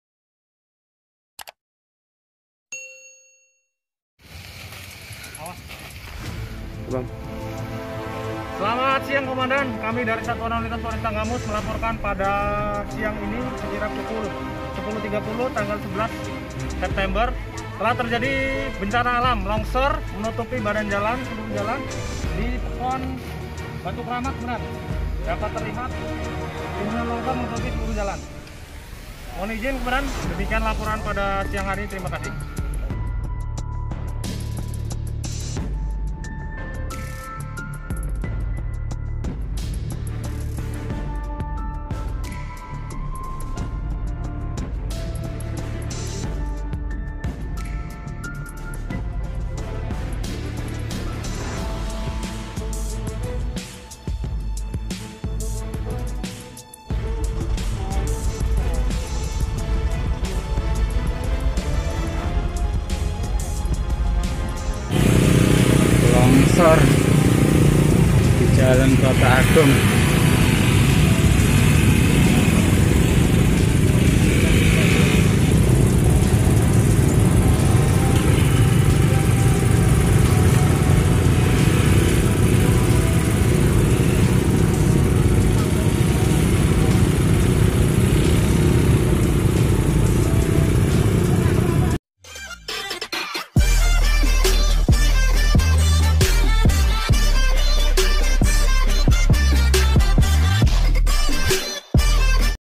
TANAH LONGSOR DIBATU KRAMAT
Telah terjadi tanah longsor di batu Kramat kota agung timur tanggamus Lampung.
Tanah longsor tersebut menutup jalan lintas barat.sementara hanya sepeda motor yang bisa melalui jalan tersebut